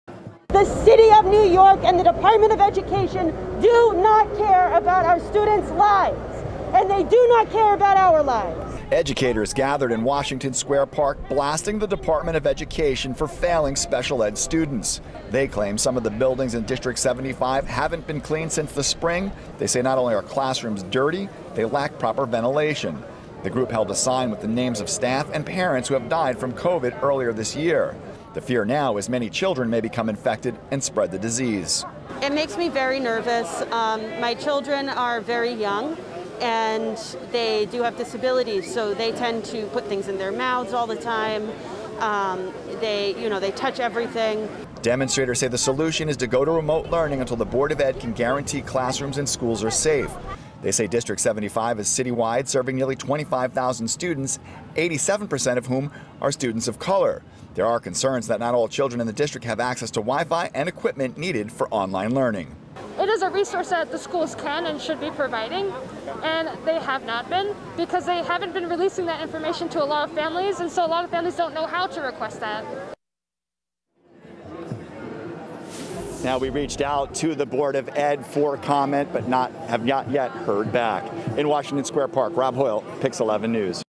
Parents protest on behalf of special needs students before school reopenings
There was a protest in Washington Square Park Saturday. Educators saying the reopening of schools for children with disabilities is unsafe and downright unsanitary.